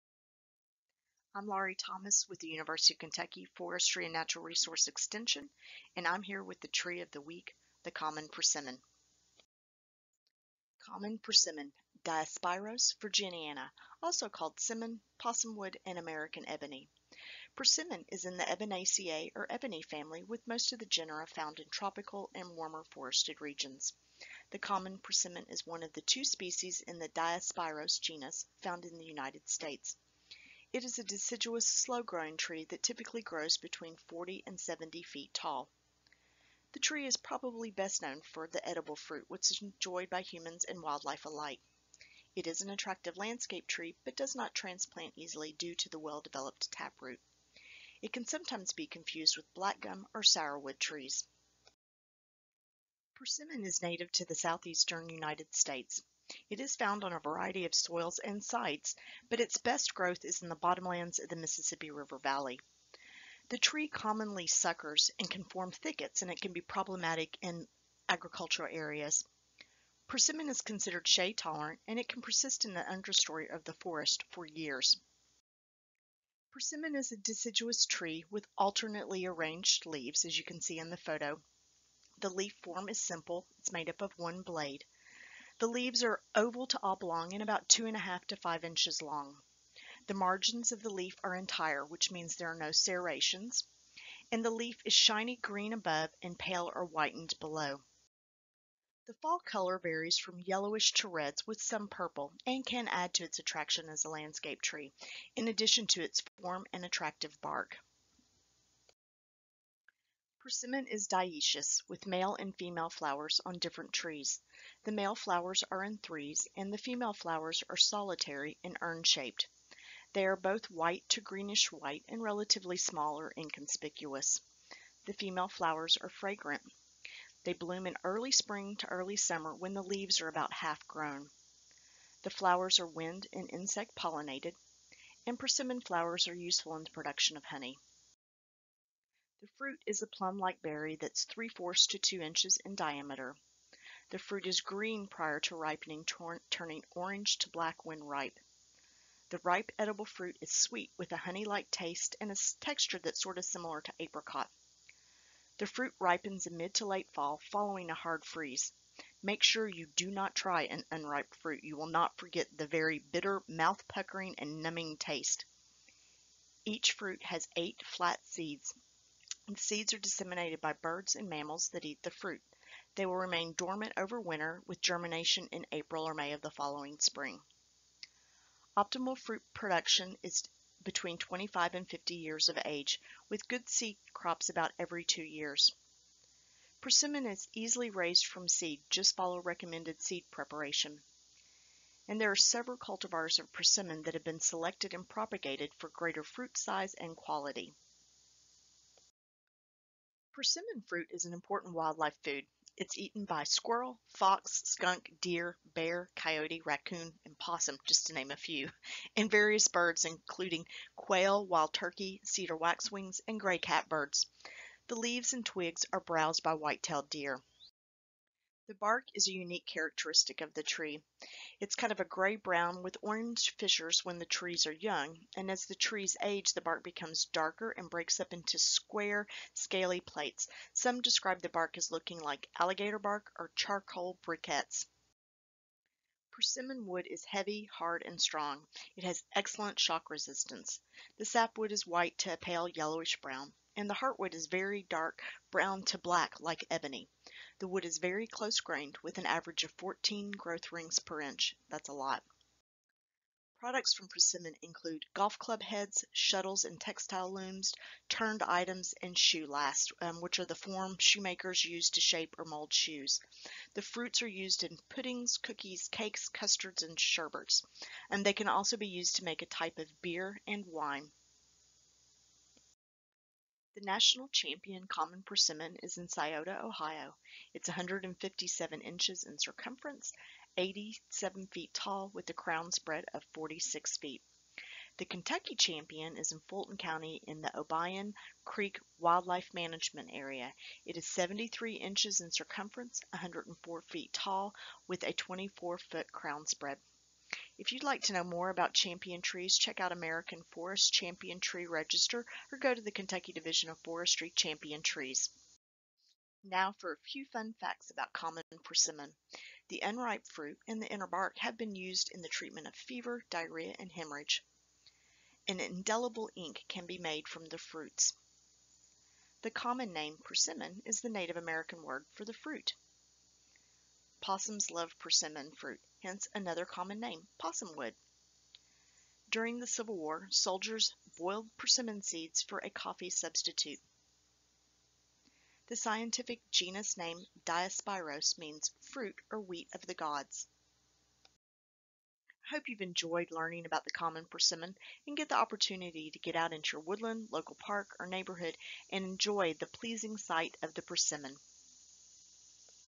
Tree of the Week: Persimmon
University of Kentucky Department of Forestry's Extension Forester Laurie Thomas describes to us the persimmon tree.